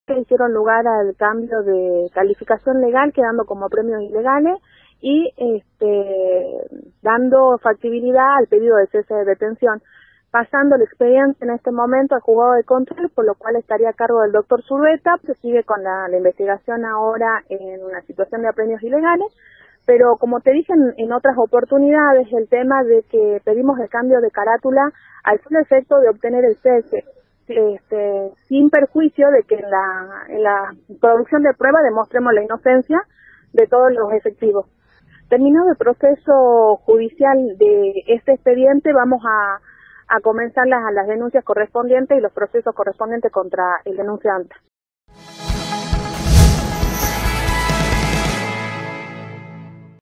Cambio de carátula y cese de detención para los cinco policías de Perico
Lo confirmó Raquel Almazán, abogada defensora de los detenidos. Recuperarían su libertad esta tarde, y volvería a cumplir sus funciones en la fuerza policial.